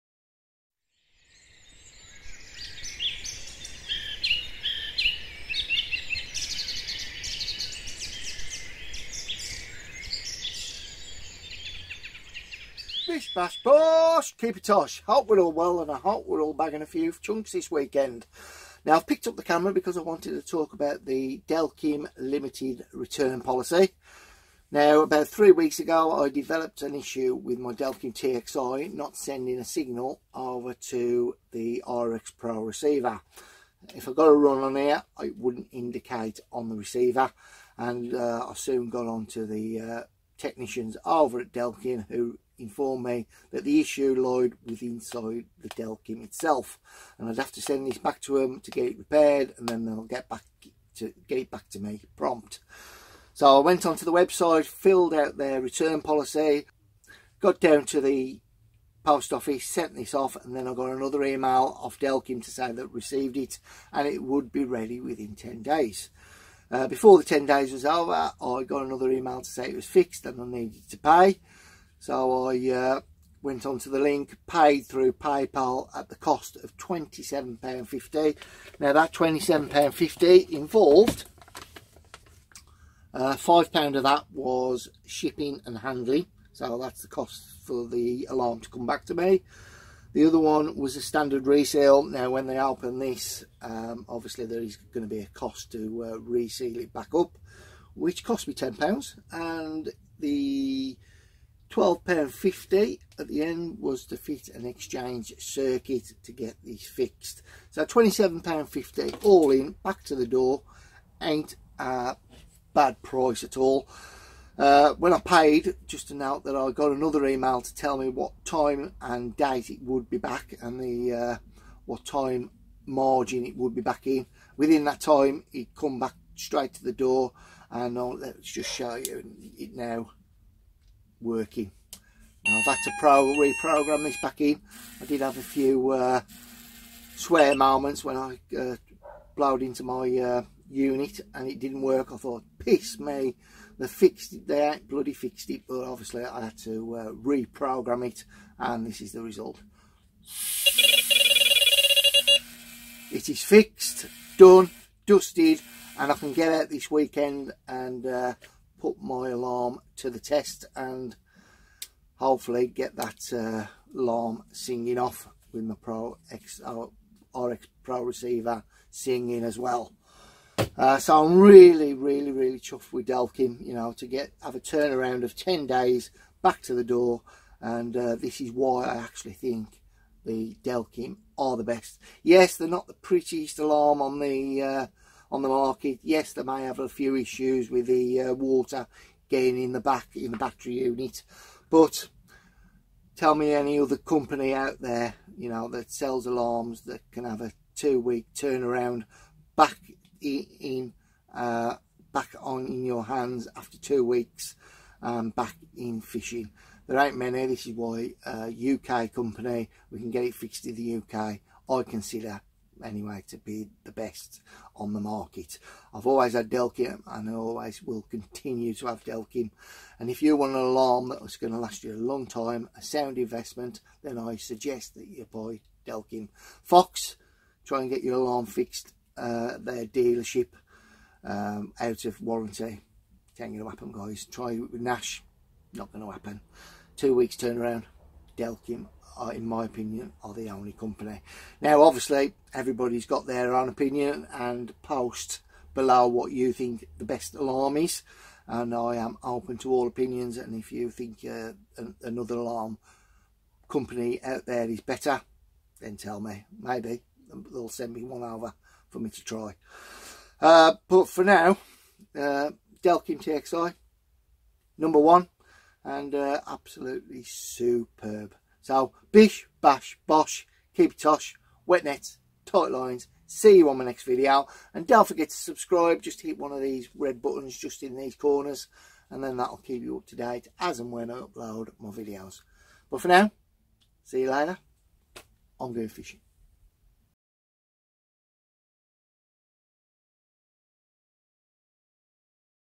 DELKIM LTD RETURN POLICY !! *CARP FISHING*
So my bite Alarm broke ? So why do i still rate Delkim as the Best ???